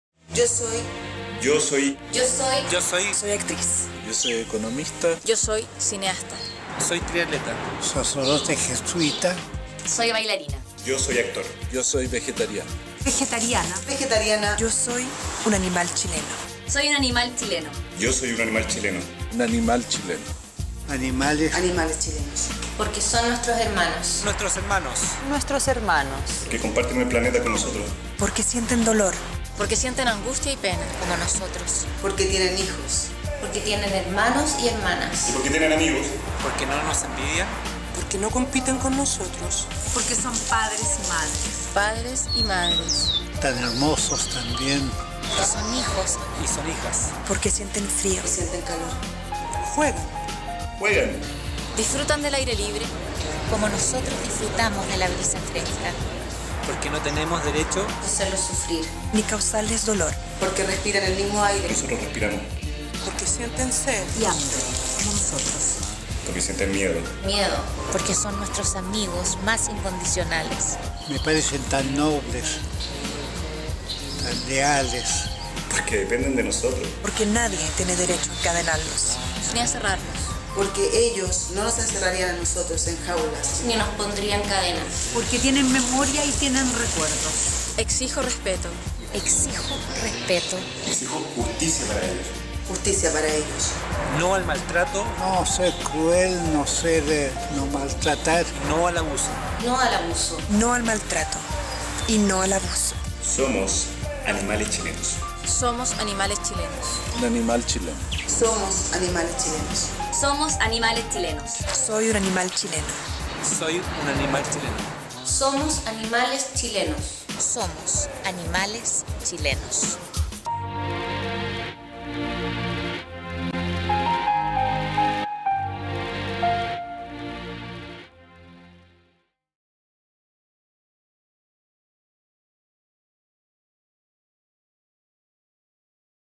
Soy un animal chileno.
Leonor Varela se suma al team original de Animales Chilenos.